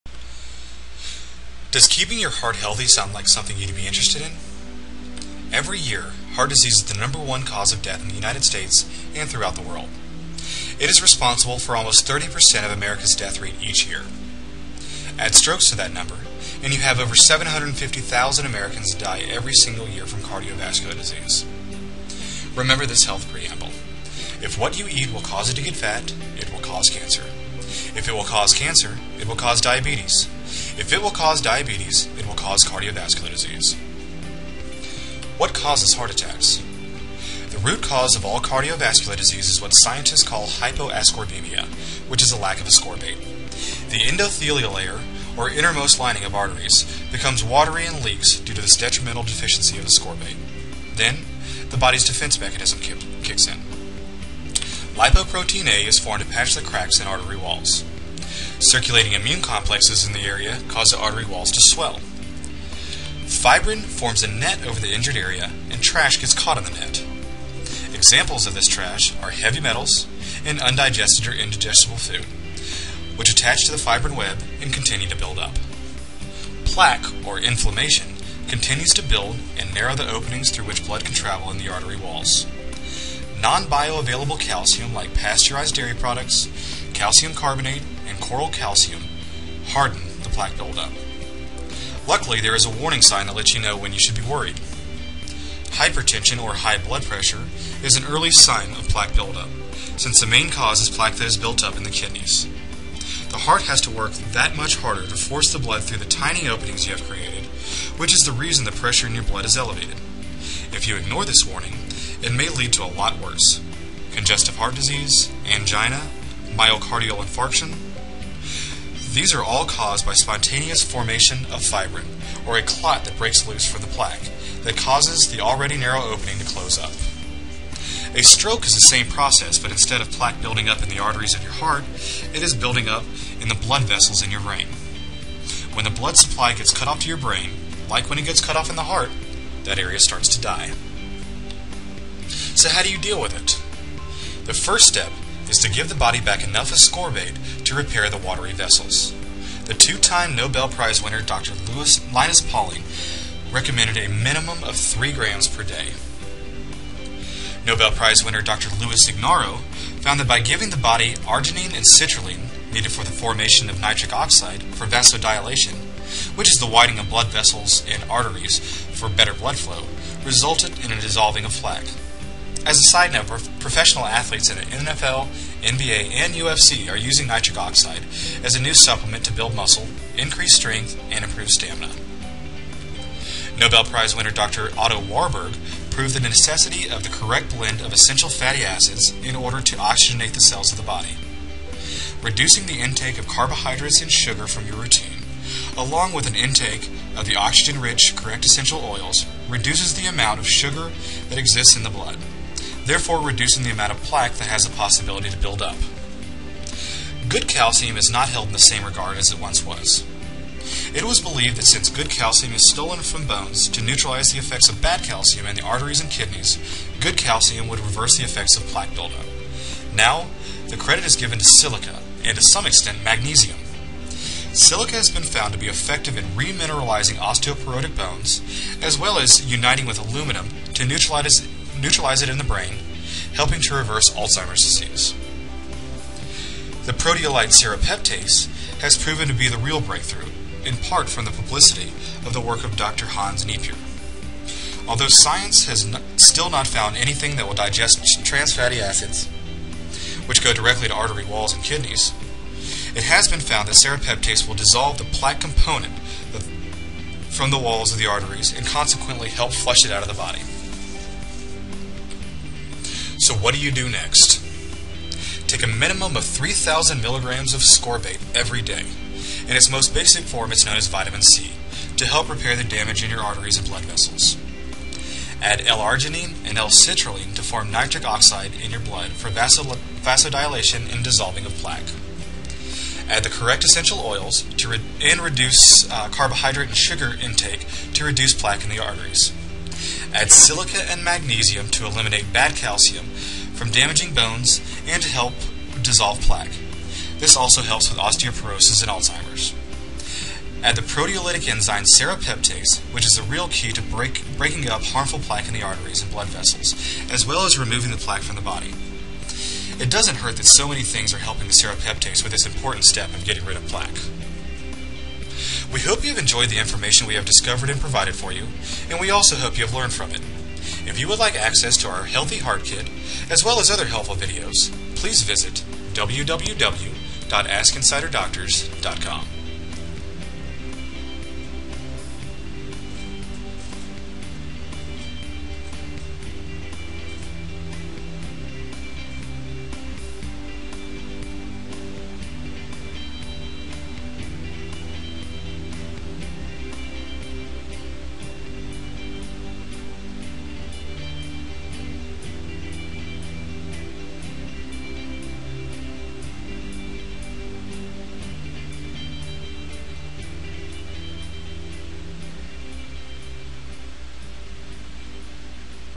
Healthy Heart - The Single Underlying Cause of ALL Cardiovascular Disease
Learn how to help keep your arteries free of plaque to prevent high HP, strokes, and heart attack.